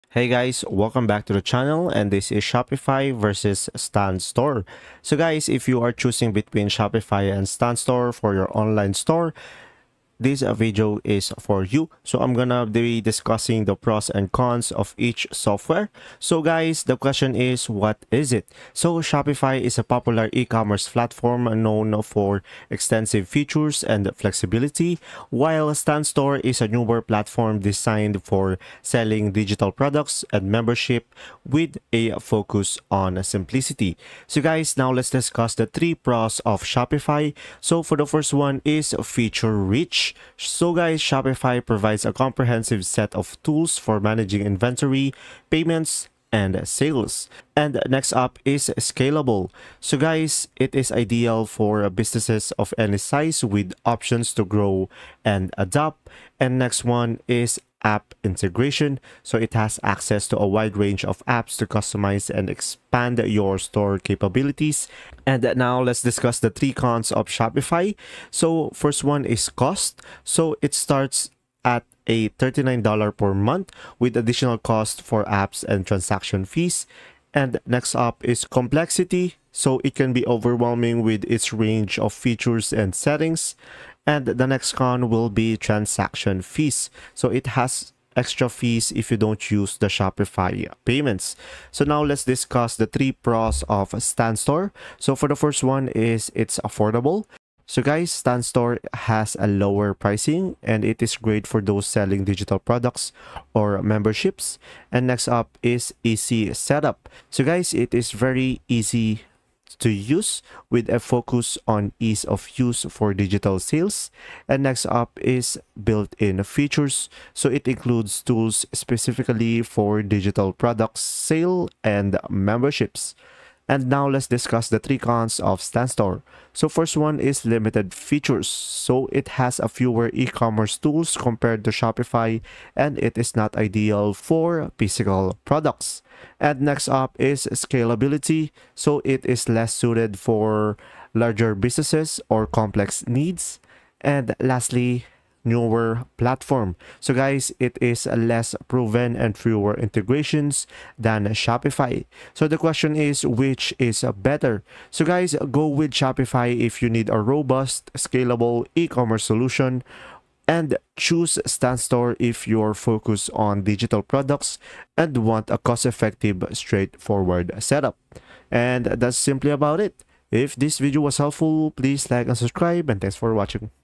Shopify vs Stan Store (2024) - Don't Waste Money!
Trying to decide between Shopify and Stan Store for your online business? This 2024 comparison breaks down their features, pricing, pros, and cons to help you choose the best platform for your e-commerce or personal brand store.